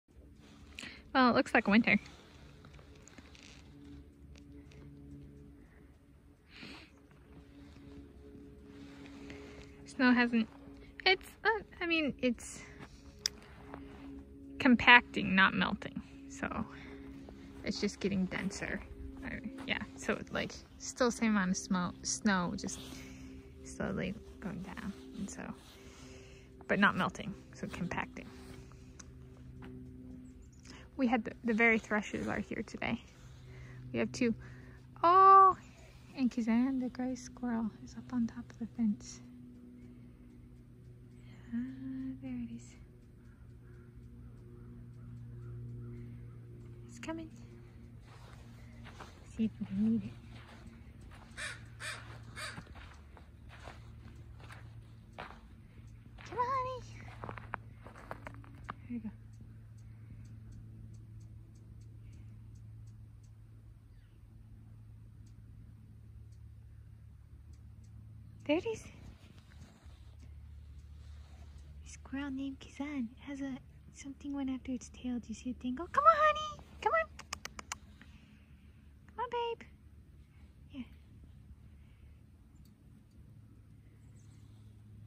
Kazan the Gray Squirrel out for an afternoon peanut in the snow
Nice sunny afternoon, still below freezing in temperature though meaning the snow is compacting instead of melting, and Kazan the Gray Squirrel out for an afternoon peanut.

Filmed by me (Tanis Coralee Leonhardi) in Shoreline, Washington State, United States of America starting at 3:00 p.m. (Pacific time) on Monday 27 December 2021. Premiered 27 December 2021.